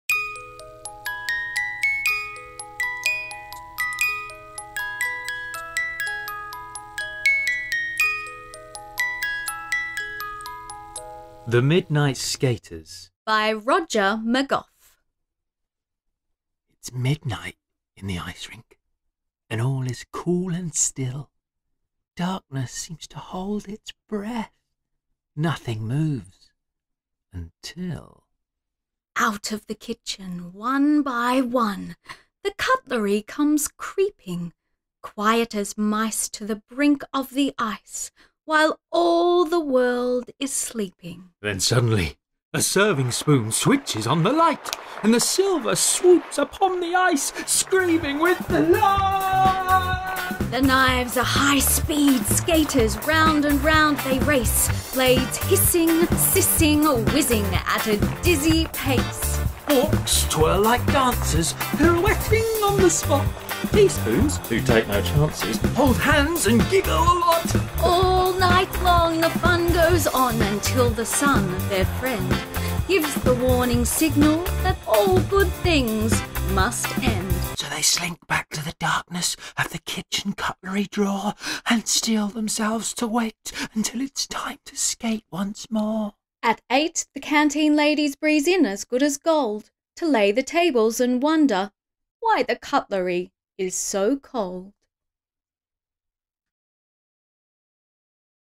The Midnight Skaters, written by Roger McGough, and read by Andrew Cullimore and Sarah Hamilton.
Made entirely for fun and 100% none commercial for Rhys and Kenzie.

Photo of Roger McGough by Colin Clarke
Photo of Andrew Cullimore and Sarah Hamilton by Adrien Rossignol